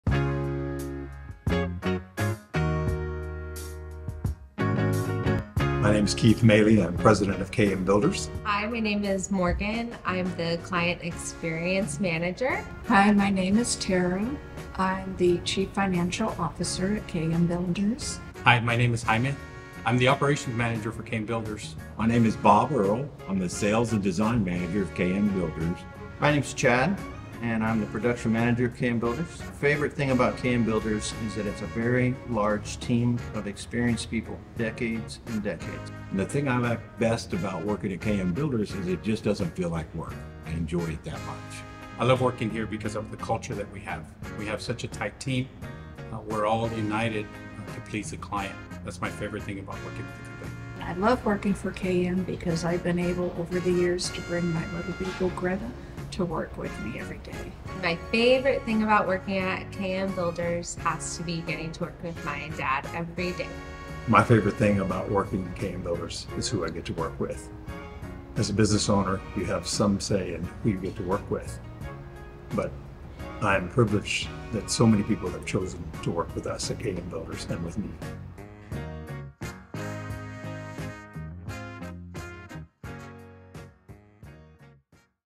Meet our Management Team!
The skills, vision and experience of our management team is what sets us apart. Meet the impressive leaders guiding our remodeling excellence.